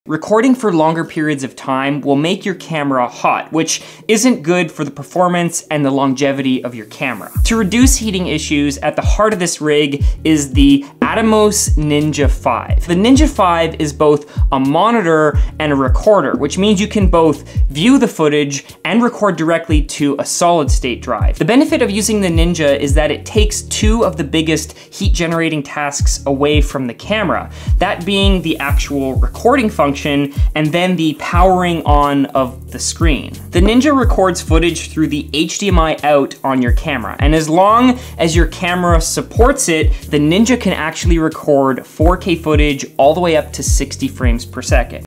Record WITHOUT OVERHEATING 🥵📷
Here's how to record video out from your camera's HDMI port to an external monitor without overheating.

BEST 4K External Recorder

📷 My Cameras and Lenses
My Camera
My Favorite Lens
Lens 1
Lens 2
Lens 3
ND Filters
My favorite memory cards

[Timecodes]
0:00 My Video Recording POWERHOUSE
0:28 Why I chose the R6 (It overheats)
1:08 Base Rig and parts overview
2:42 Why Rig out your camera?
3:02 Atomos Ninja V
4:17 Powering a camera with a USB C Power Bank
5:41 Settings for the Canon R6
6:11 How long can the camera record?
7:07 More accessories!
7:55 How to use a Matte Box?
9:07 So Professional...